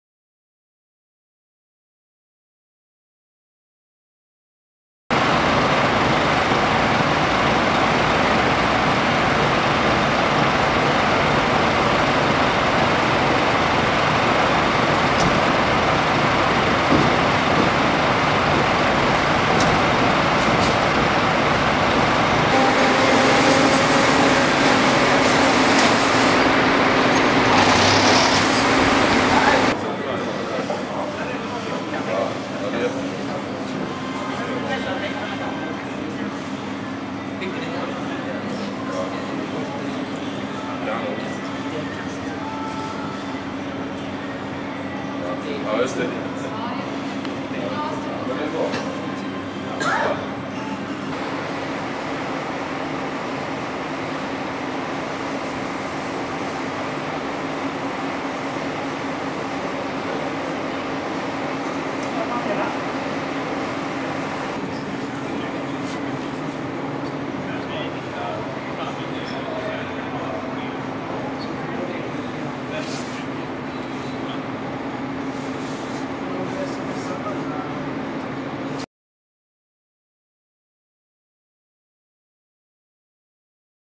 111211_BrandNordstan.mp4
En storbrand hotade affärscenrumet Nordstan i Göteborg på söndagsmorgonen. Det började brinna i en klädaffär tidigt på morgonen. Branden hotade att sprida sig tillnärliggande butiker. Tack vare en stor insats från Räddningstjänsen kunde branden snabbt slås ner. Eftersläckningsarbetet kommer att ta några timmar. Delar av Nordstan kommer att vara avspärrat under denna tiden.